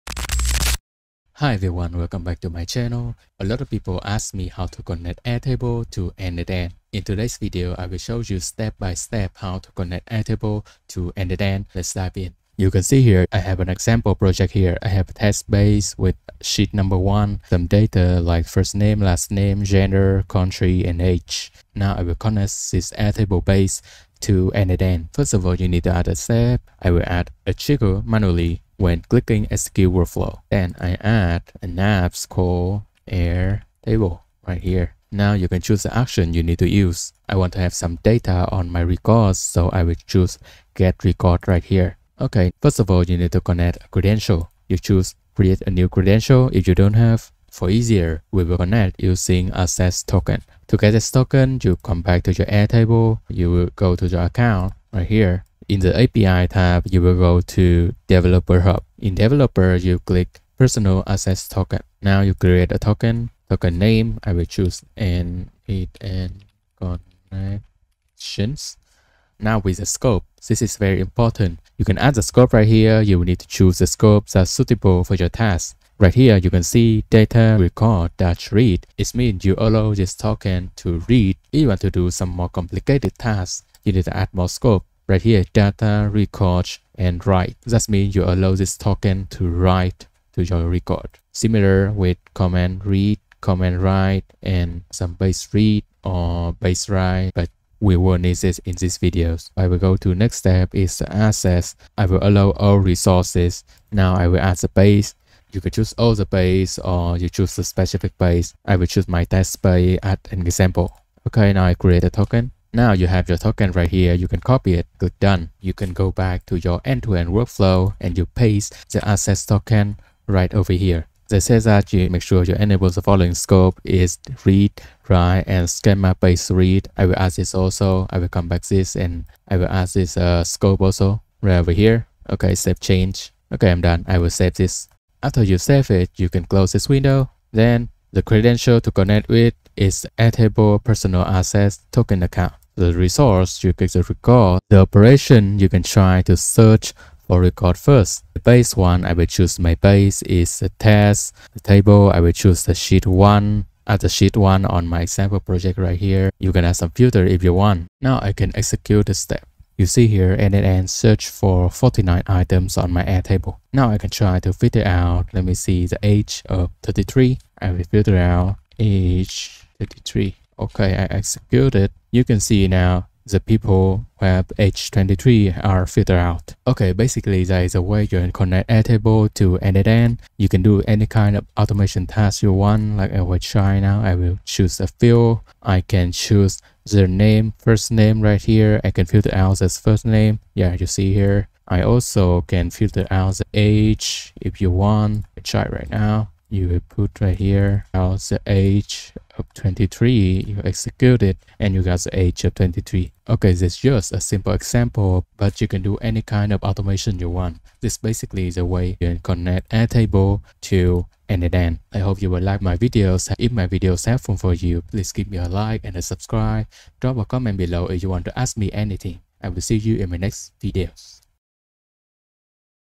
How to connect Airtable to n8n - Simple tutorials in 2025
Learn how to connect Airtable to n8n seamlessly in this simple tutorial for 2025! This video will walk you through a step-by-step process to set up powerful workflow automations and synchronize your data effortlessly between Airtable and n8n. Discover how to leverage n8n to automate tasks, move information between Airtable bases, and create efficient no-code or low-code integrations. This comprehensive guide covers everything from initial setup to advanced data handling, ensuring your Airtable and n8n workflows are optimized for productivity.